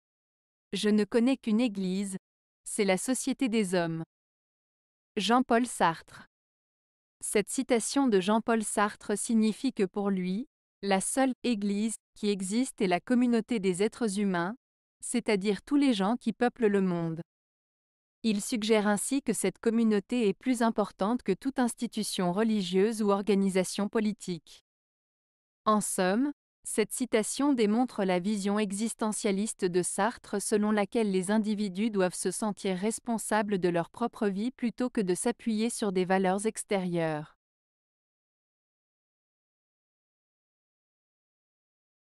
Je ne connais qu'une Eglise - c'est la société des hommes. - Jean-Paul Sartre
Cette citation de Jean-Paul Sartre signifie que pour lui, la seule "église" qui existe est la communauté des êtres humains, c'est-à-dire tous les gens qui peuplent le monde. Il suggère ainsi que cette communauté est plus importante que toute institution religieuse ou organisation politique. En somme, cette citation démontre la vision existentialiste de Sartre selon laquelle les individus doivent se sentir responsables de leur propre vie plutôt que de s'appuyer sur des valeurs extérieures.